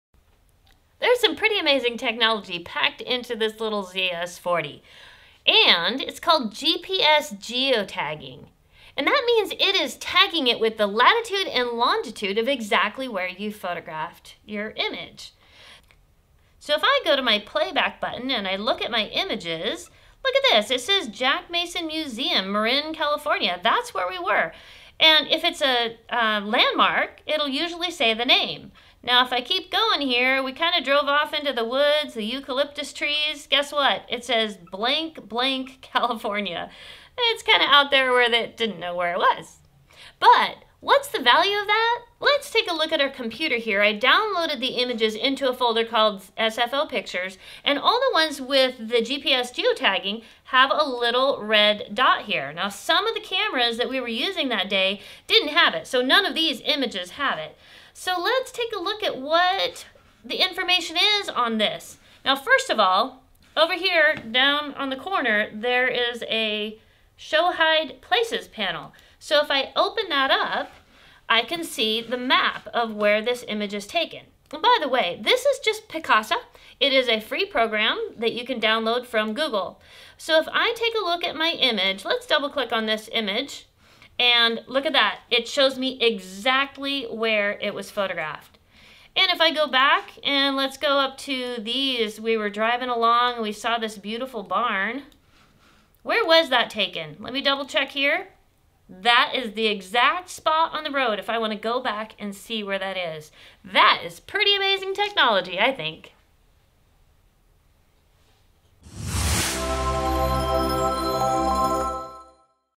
Panasonic - LUMIX Point and Shoot - DMC-ZS40 - How to use the GPS function.
The GPS (Global Positioning System) in the DMC-ZS40 complies with GLONASS (Global Navigation Satellite System) to further enhance its detection accuracy for its increase number of satellites.  The GPS function allows you to tag your images and video. When viewed in a program such as Picasa by Google, you can see the location on a map.  In this video we will show you how to use the GPS tag functions.